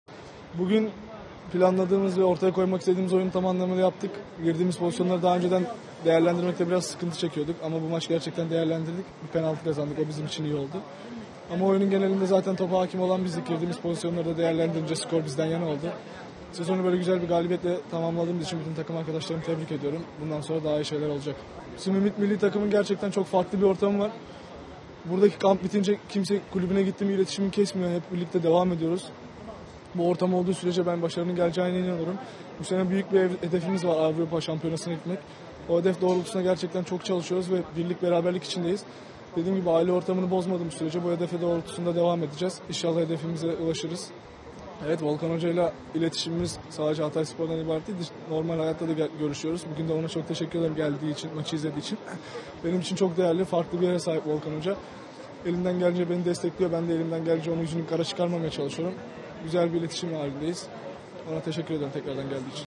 Bertuğ Yıldırım: Avrupa Şampiyonası hedefimiz doğrultusunda...
Ümit Milli Takımı’nın başarılı futbolcusu Bertuğ Yıldırım, kamp dönemini galibiyetle tamamladıkları için çok mutlu olduğunu söyleyerek, Avrupa Şampiyonası hedefi doğrultusunda çok çalıştıklarını söyledi. Yıldırım, Bosna Hersek maçını izlemeye gelen Hatayspor Teknik Direktörü Volkan Demirel için ise, "Volkan Hoca ile iletişimimiz sadece Hatayspor’dan ibaret değil. Bugün ona da çok teşekkür ederim geldiği ve maçı izlediği için. Benim için çok değerli, farklı bir yere sahip" dedi.